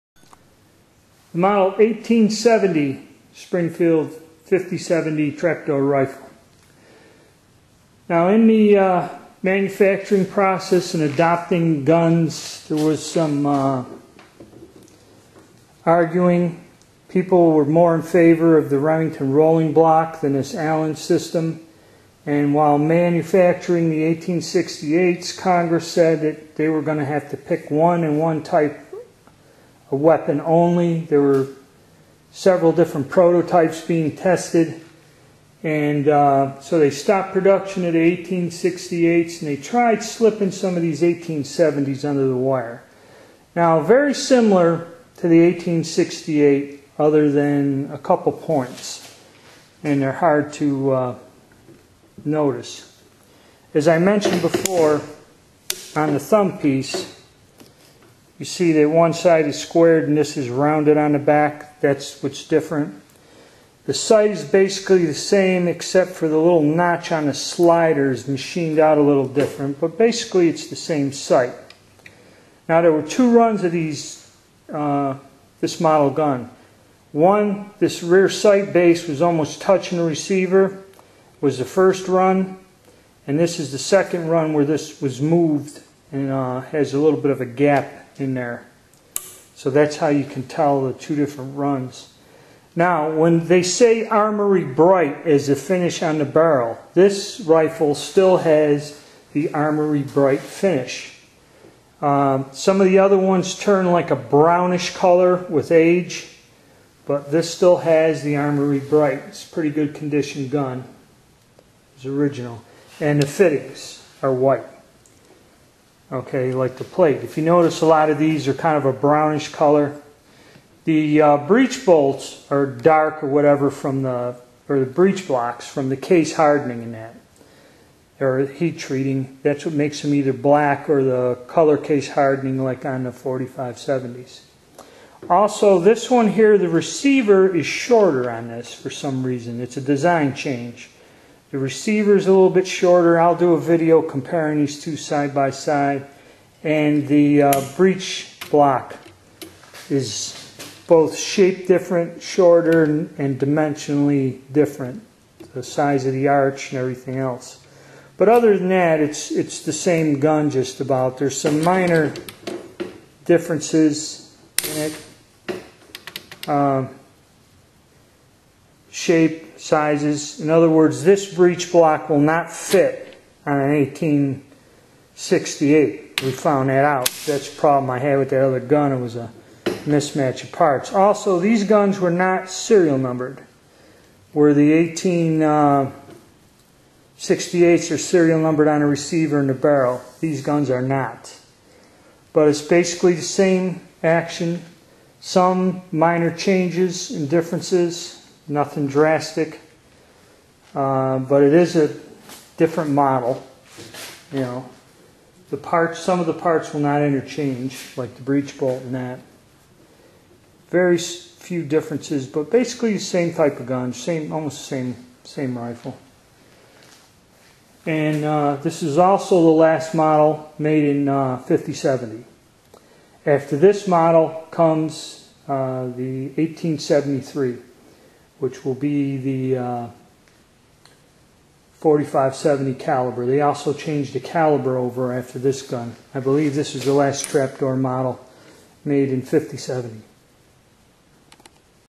US 1870 50-70 Springfield Trapdoor Rifle
A quick overview of the springfield 1870 trapdoor rifle